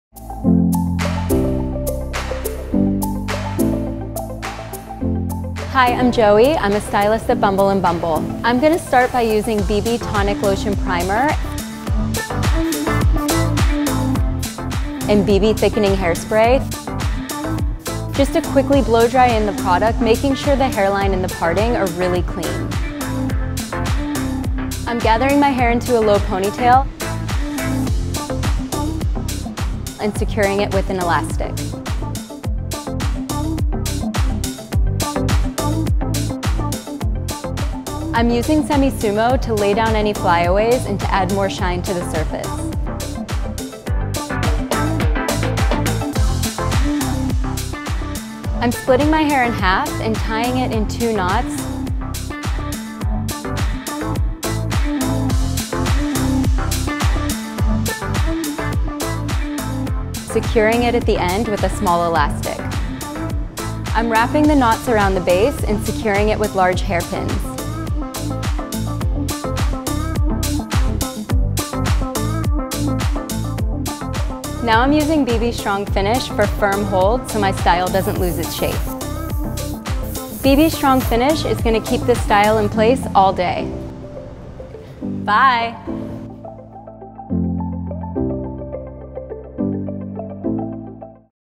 Bumble and Bumble Knotty Bun Tutorial | bluemercury
Bumble and bumble presents the Knotty Bun with Bb. Strong Finish Spray. Joey demonstrates how to achieve the perfect knotty bun using Bumble and bumble's Strong Finish Spray.

How to create a knotty bun hairstyle with Bumble and Bumble's strong finish hair spray.

A high-perfomance aerosol hairspray that provides strong, firm hold with model finish. Ie. No crunch!

BUMBLE AND BUMBLE
strong finish hairspray